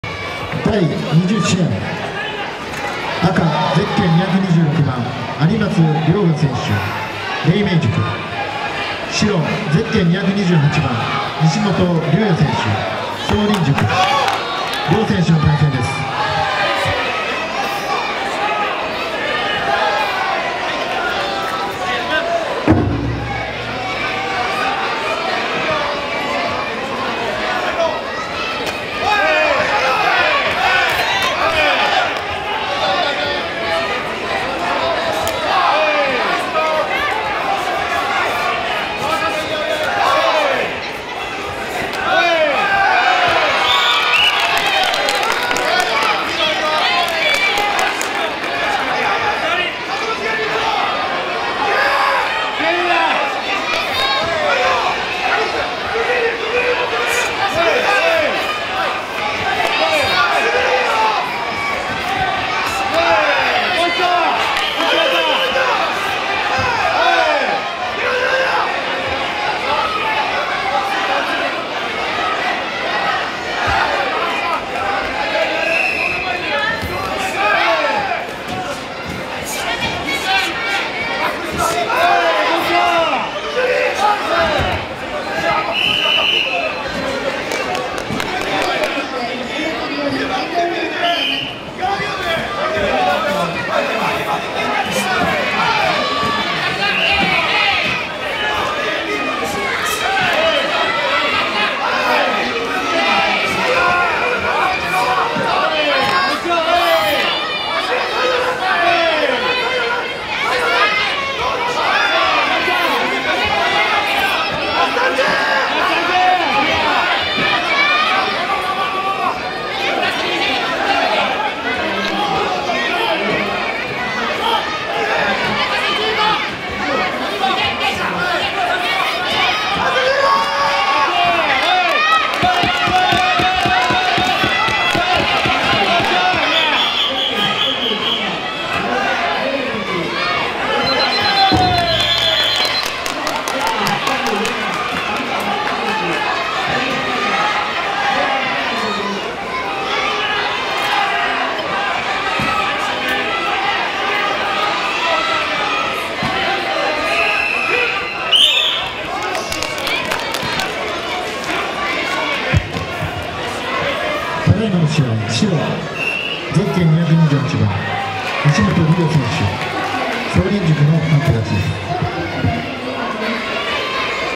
【白蓮会館】有松 稜我 (黎明塾)vs 西本 竜也 (叢林塾) 〜全九州空手道選手権大会
第13回 ファイティングオープントーナメント
全九州空手道選手権大会
一般軽量級 (高校生以上75kg未満)

226 有松 稜我 (黎明塾)
228 西本 竜也 (叢林塾)

2017年 6月18日
佐賀大和勤労者体育センター
主催：白蓮会館九州本部

白蓮会館九州本部本部長・大野英章のブログ